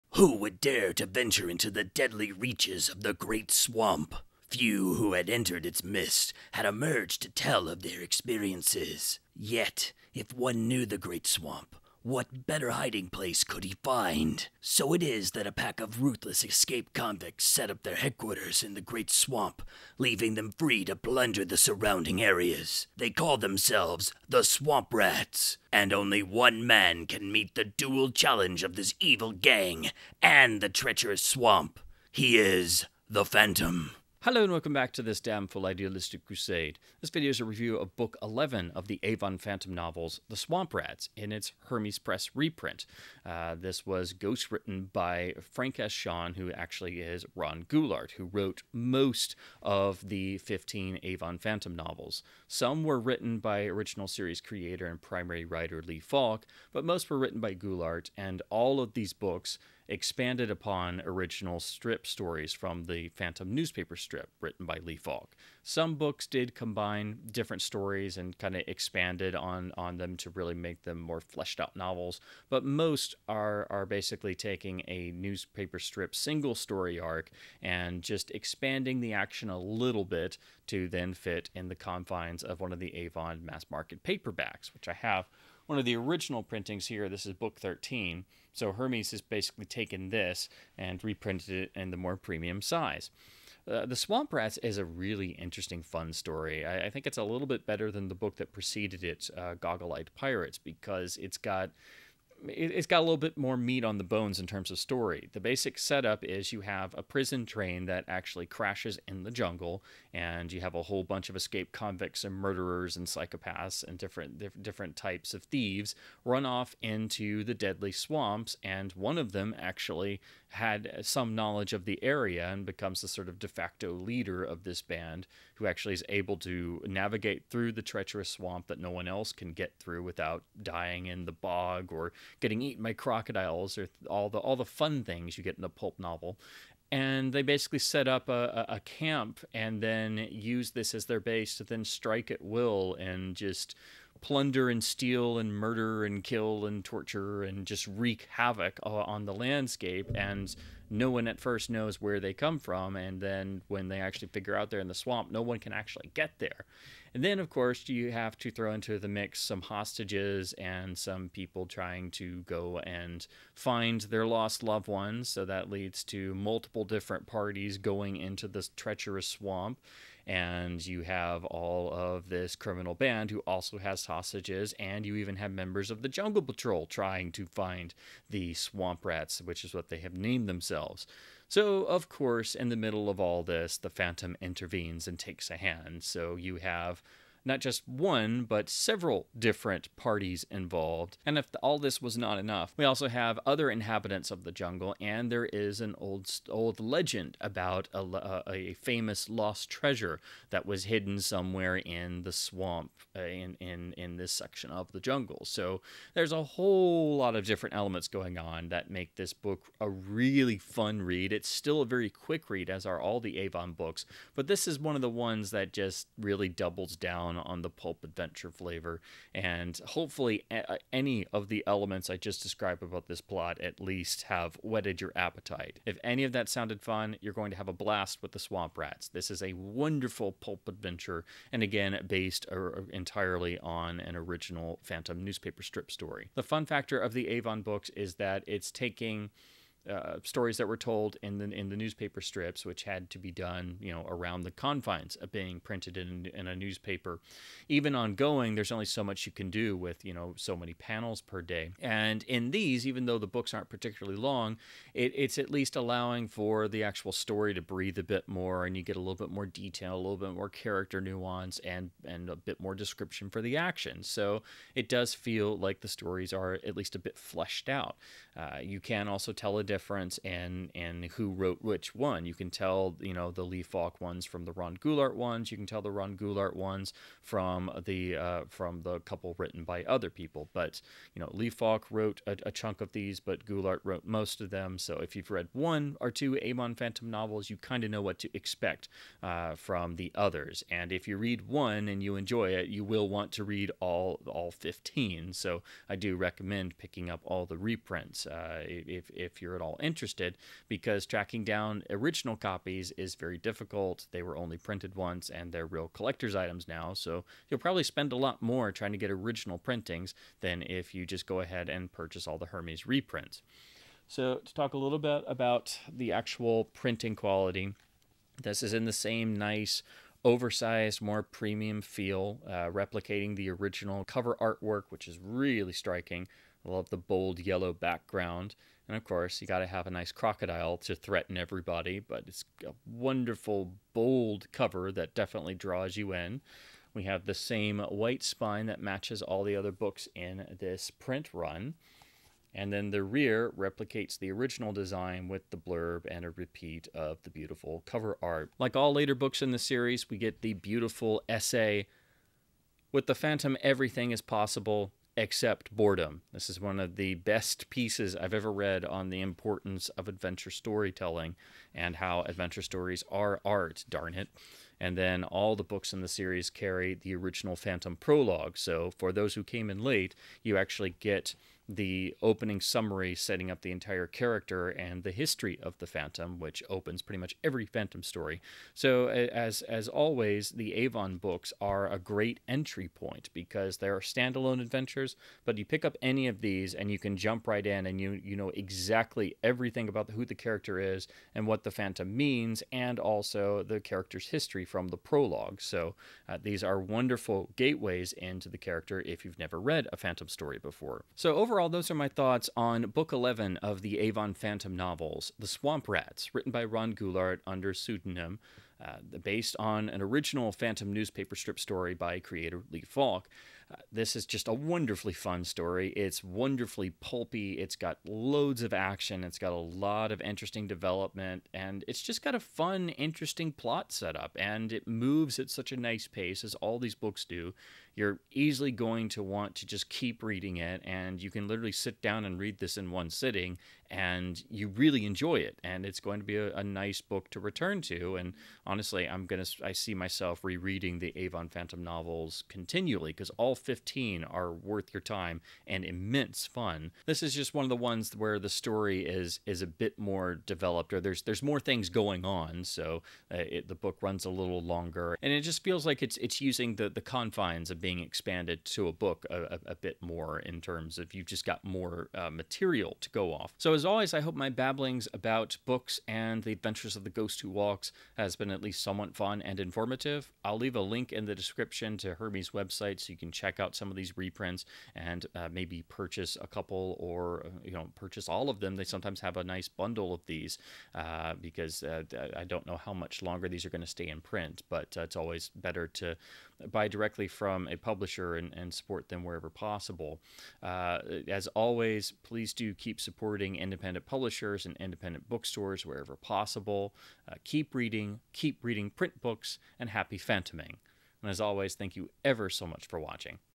Avon The Phantom Novels # 11 THE SWAMP RATS by Ron Goulart BOOK REVIEW
My review of the 11th Avon novel based on The Phantom, THE SWAMP RATS by Ron Goulart.
Or pick up a discount bundle of all 15 Hermes Avon Phantom reprints